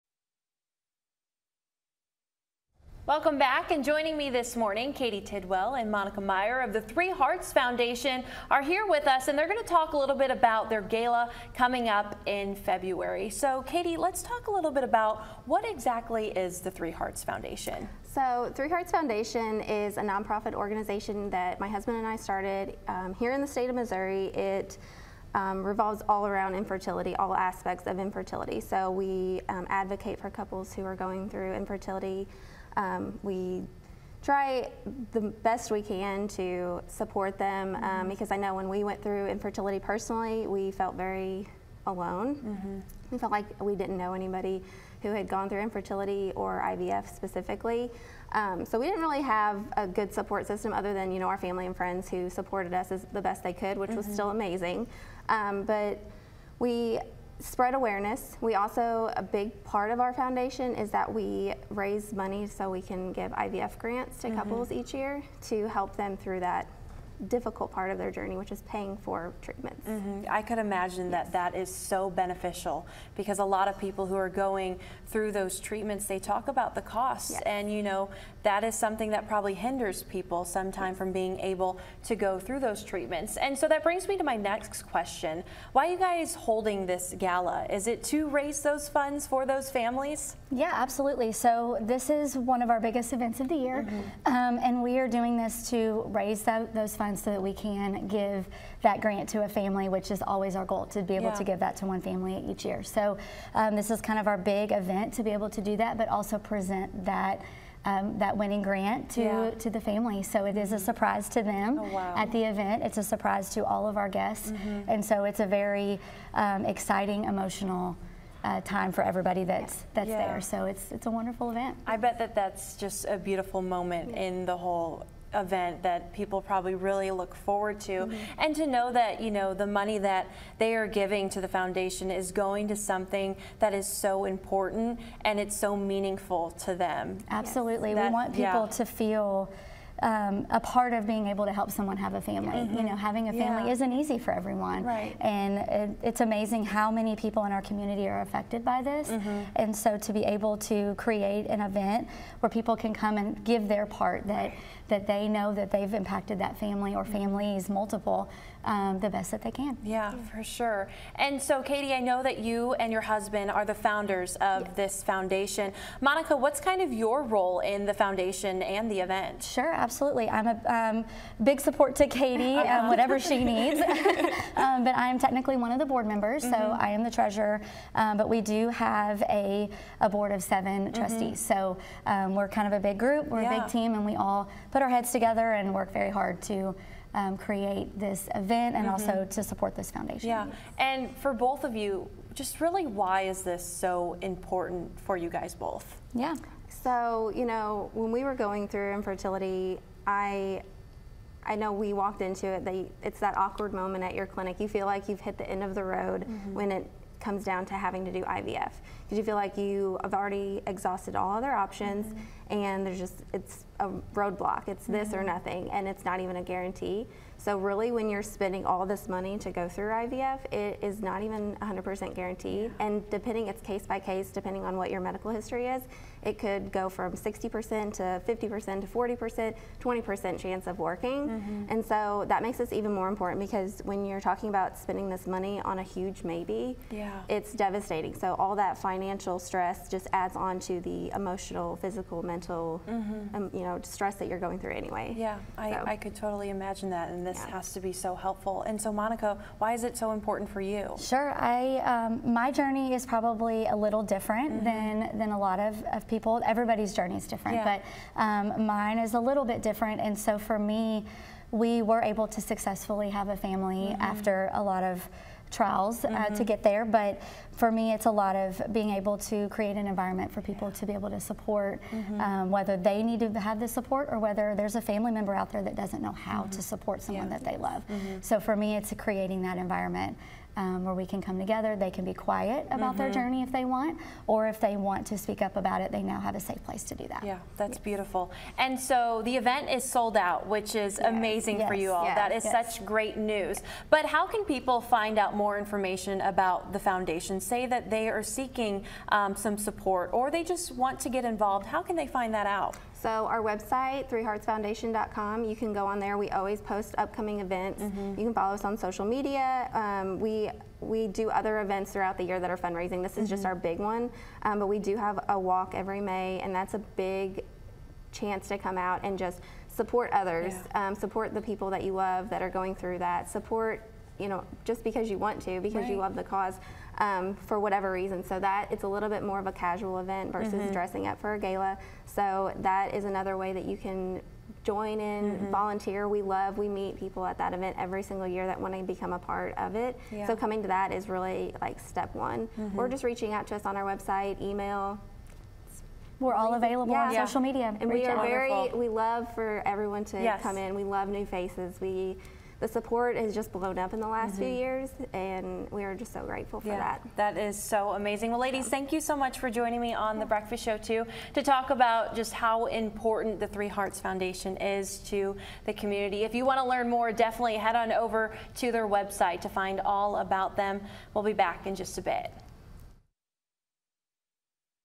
Heartland event set to raise awareness & support for couples dealing with infertility
Katie Tidwell and Monica Meyr, of Three Hearts Foundation, talk about their gala coming up in February.